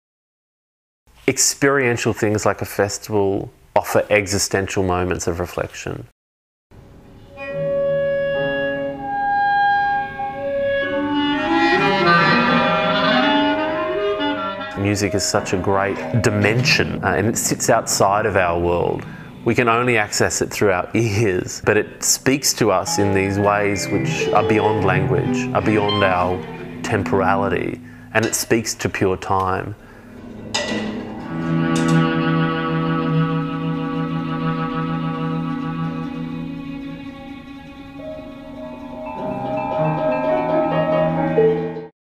BIFEM - The Bendigo International Festival of Exploratory Music - 2014 Trailer
As a city founded on exploration and discovery, and with a spirit of independence, openness to new ideas, and with a people recognised for their generosity and grace,  Bendigo proved itself the perfect home for the new festival in 2013.

The success of its inaugural edition sees the Bendigo International Festival of Exploratory Music return in September 2014.

BIFEM 2014 will see an expanded programme of concerts, recitals, performance-lectures, workshops and special events - 22 across the four days of this free and low-cost music festival. Featuring an even larger number of international guests and remarkable composers many of whom have never before been performed in Australia, our hallmarks of virtuosity and  innovation remain available to you for the about the price of a cinema ticket.

For the many of you who regretted missing 2013, be sure to put September 4-7 in your calendars.

See you next spring!

David Chisholm
Founder and Artistic Director, BIFEM